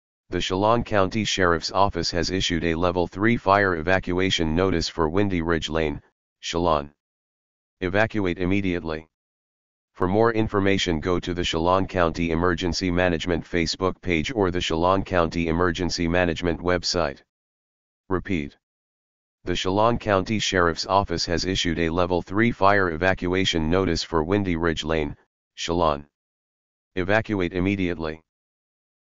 Evacuation Immediate for Wildfire for Windy Ridge Lane in Chelan County, WA - 2:57 PM PDT 8/28/22
Evacuation Immediate for Wildfire issued by Chelan County Sheriff's Office at 2:57 PM PDT 8/28/22 for Chelan County [WA]. WARN ID: a884a819